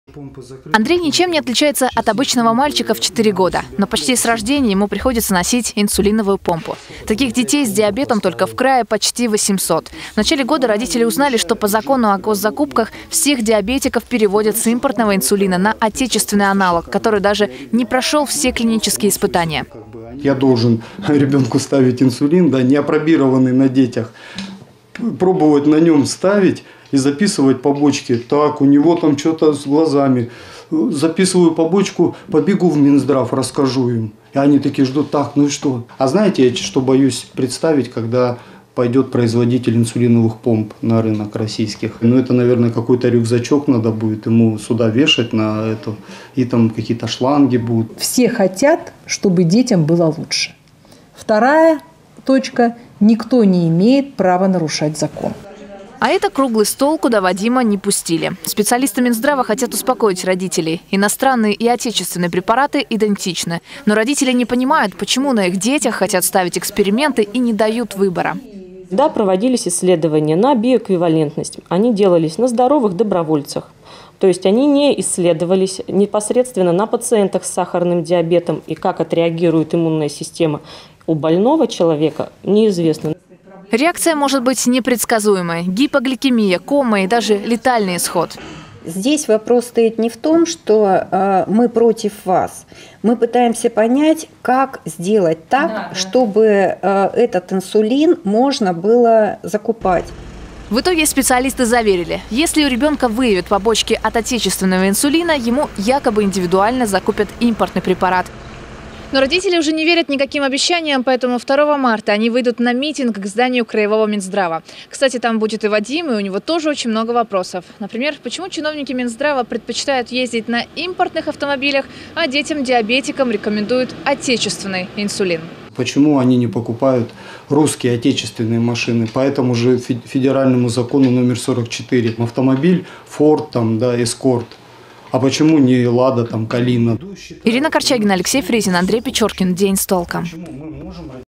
Опыты на детях? Родители детей-диабетиков Барнаула выйдут на митинг против отечественного инсулина
Родители детей-диабетиков выйдут на митинг к зданию краевого Минздрава. Причина в том, что недавно всех детей в стране начали переводить с импортного на отечественный инсулин, который даже не прошёл всех этапов исследований. Митингущие хотят, чтобы в регионе продолжалась закупка зарубежного препарата, ведь их дети – не кролики для опытов.